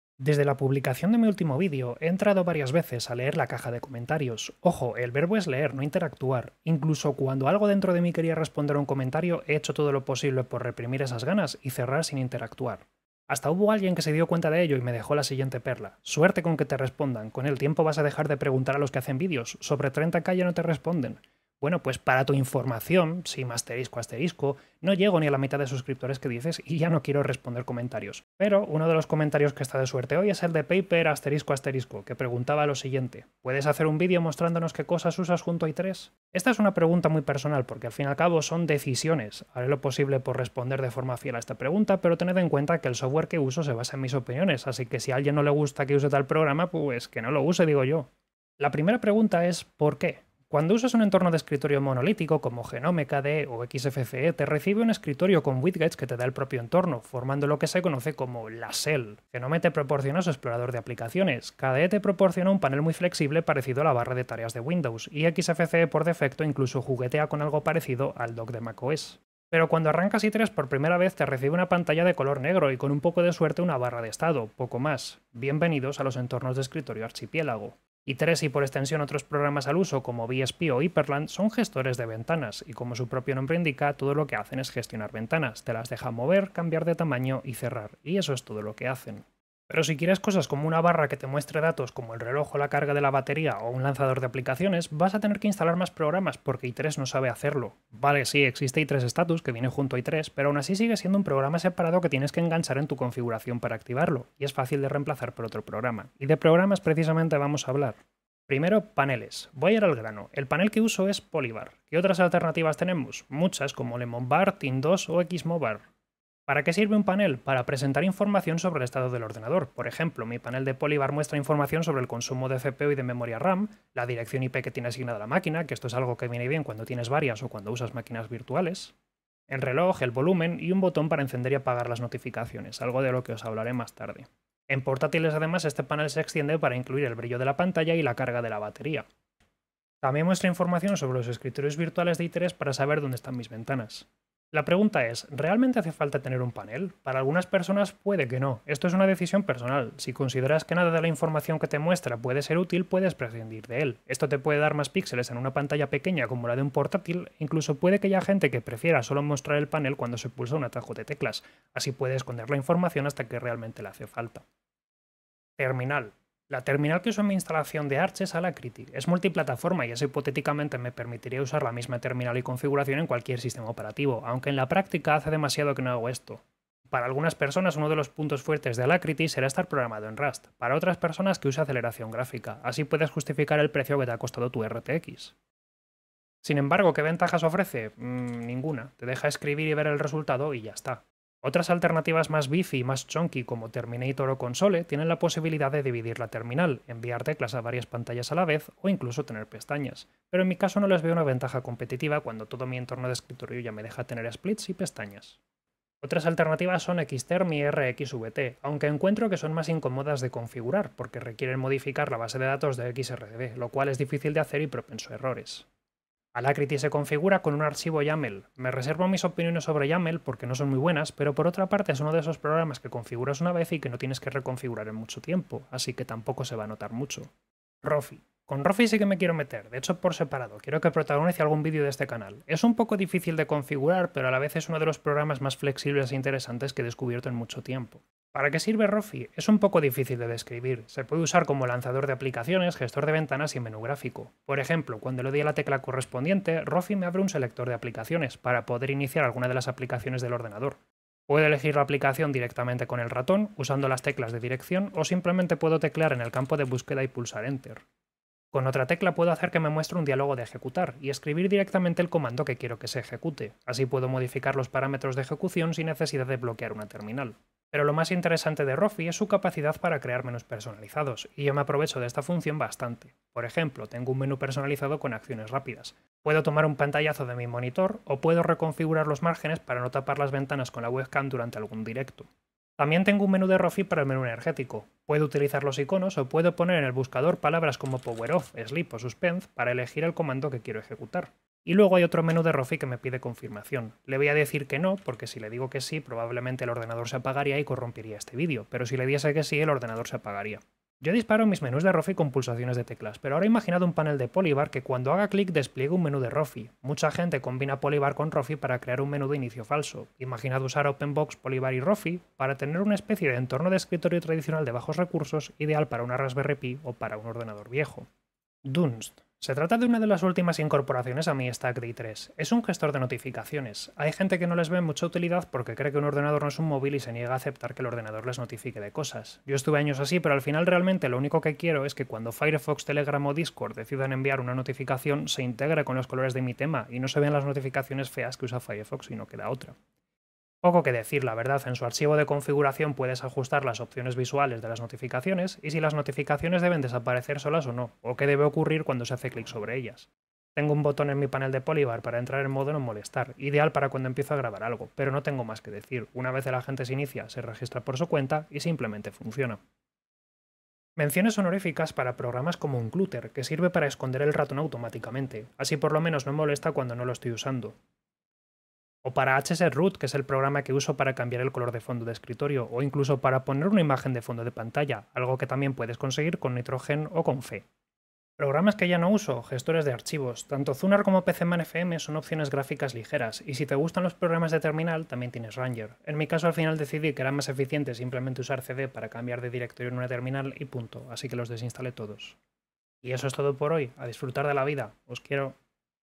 Programas que instalé junto a i3wm
Resumen: rofi, polybar, dunst, alacritty, unclutter, i3lock-fancy y hsetroot.